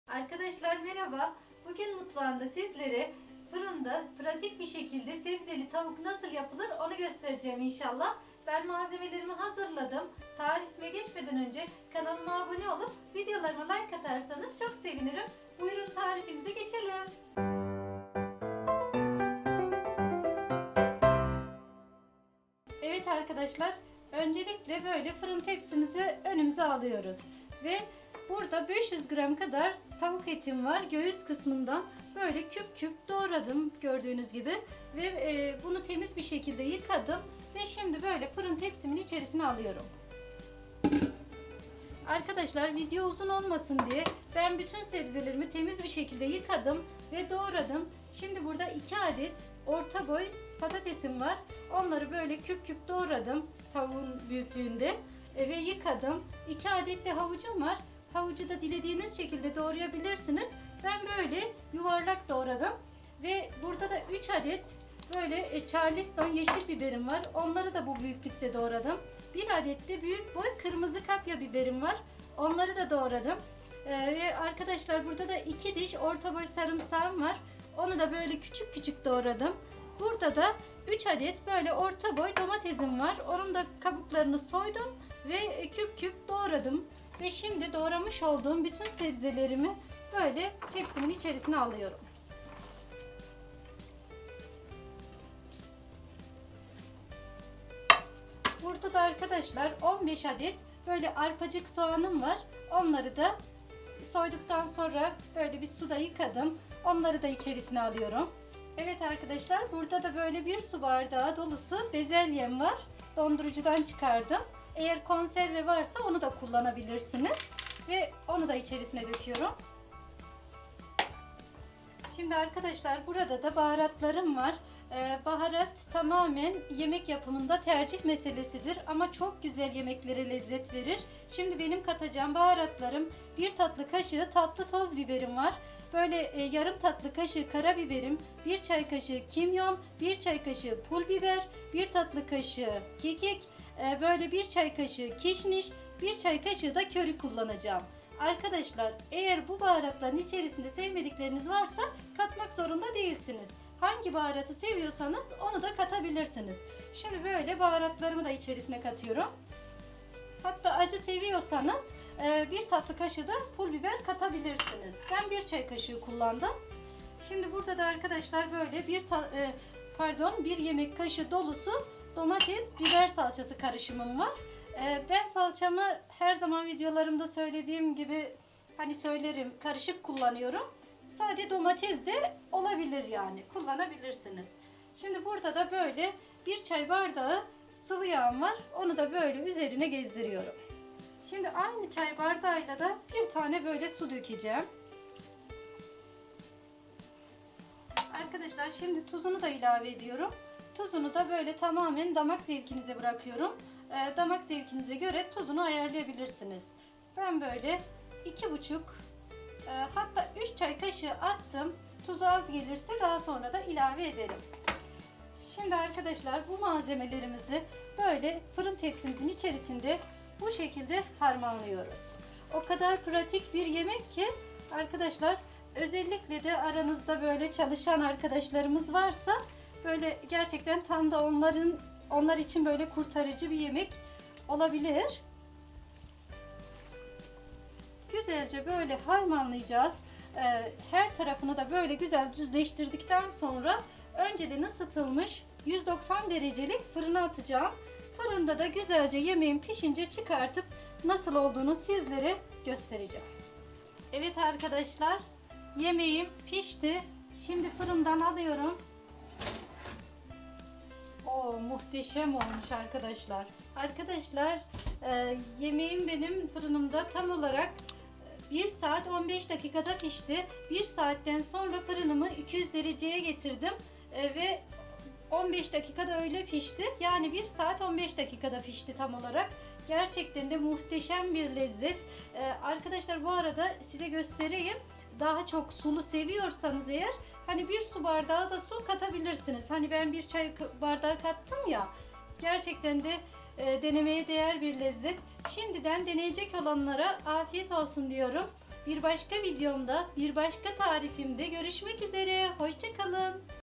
FIRINDA ÇOK KOLAY TAVUKLU SEBZE YEMEĞİ--ÇOK KOLAY TARİFLER | LEYLA ŞAHİN
MALZEMELER
500 gr tavuk eti (göğüs kısmından)
1 Su bardağı bezelye (dondurulmuş)
2 Adet orta boy patates
3 Adet orta boy domates (kabukları soyulmuş)
2 Adet orta havuç
3 Adet orta boy çarliston biber
1 Adet büyük boy  kırmızı kapya biber
2 Orta diş sarımsak
15 Adet arpacık soğan
1 Küçük çay bardağı sıvı yağ
1 Çay  bardağı Su veya 1 Su bardağı
1 Yemek kaşığı dolusu domates biber salçası
3 Çay kaşığı tuz (  Damak tadıza göre ayarlayabilirsiniz
1 Tatlı kaşığı kekik
1 Tatlı kaşığı tatlı toz biber
Yarım tatlı kaşığı karabiber
1 Çay kaşığı pulbiber
1 Çay kaşığı kimyon
1 Çay kaşığı toz kişniş
1 Çay kaşığı köri